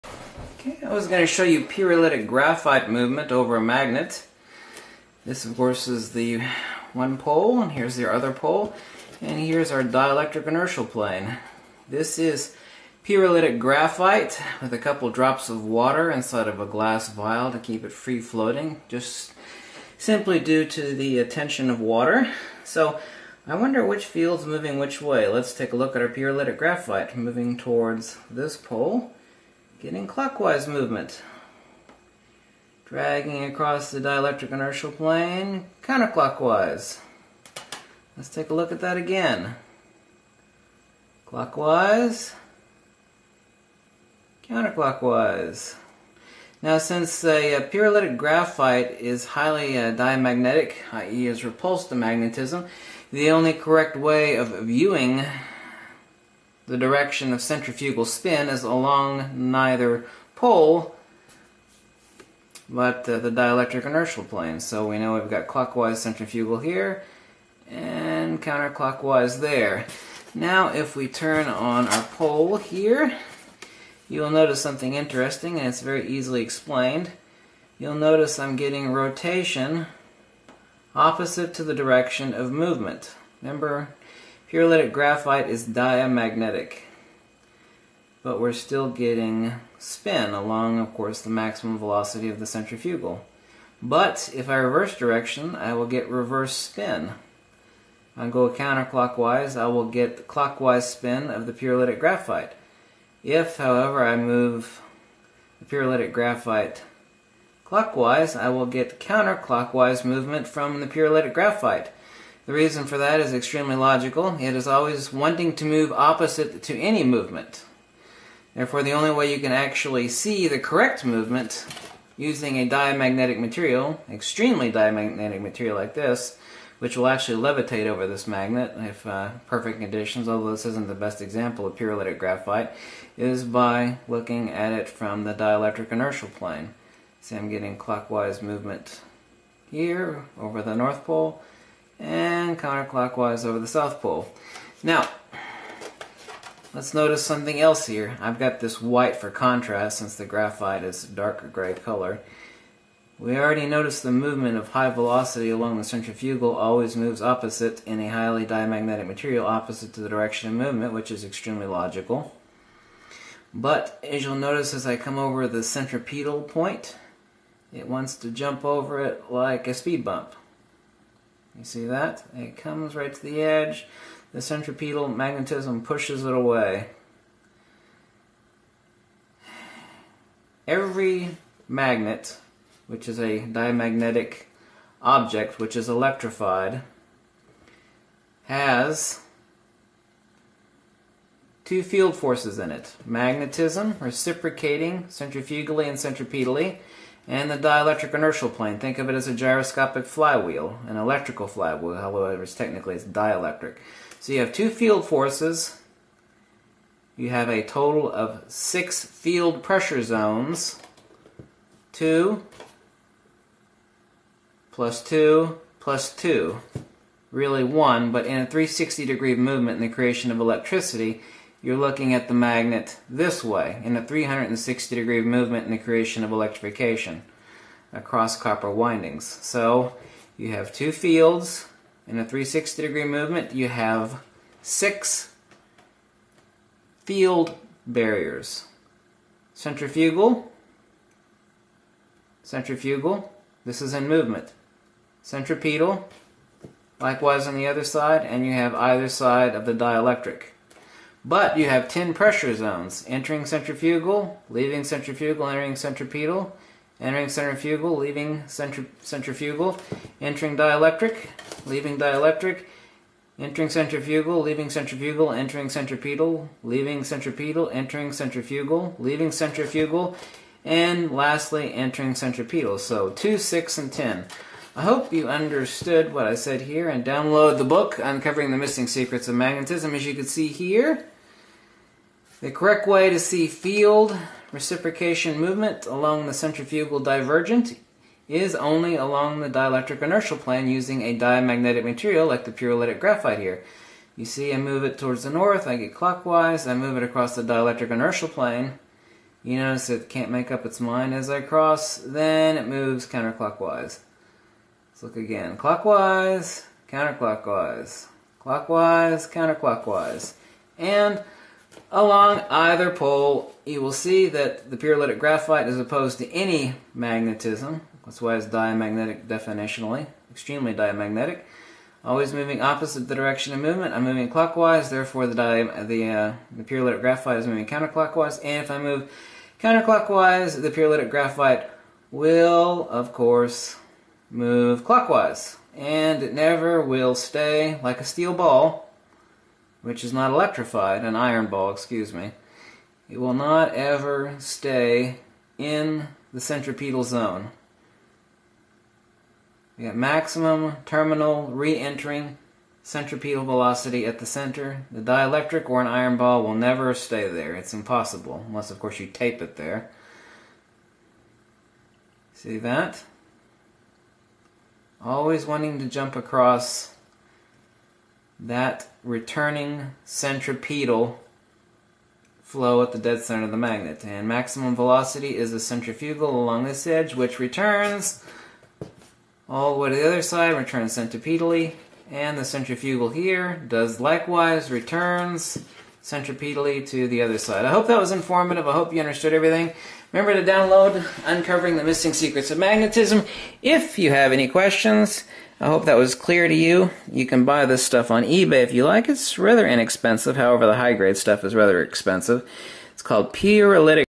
VIDEO 12 Uncovering the Missing Secrets of Magnetism, pyrolytic graphite showing Magnetic Vortex
!*!*!*!*!*!* THIS VIDEO IS A MUST SEE *!*!*!*!*!*!*

3rd EDITION due out next month VIDEO 12 Uncovering the Missing Secrets of Magnetism, Magnetic Vortex